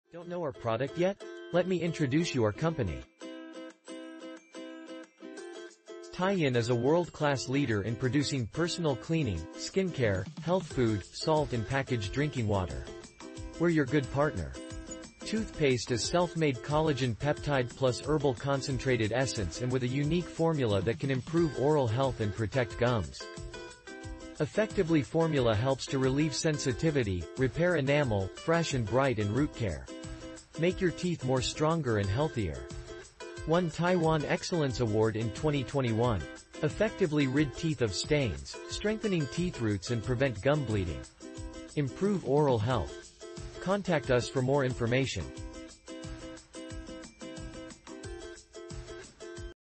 Herbal Collagen Toothpaste~Improve Oral Health & Protect Gums, Taiyen
Taiyen Collagen Herbal Toothpaste combines self-made collagen peptide (for strong gums), HAP (enamel repair), and herbal concentrated essence (soothing), with a unique formula that can improve oral health and protect gums.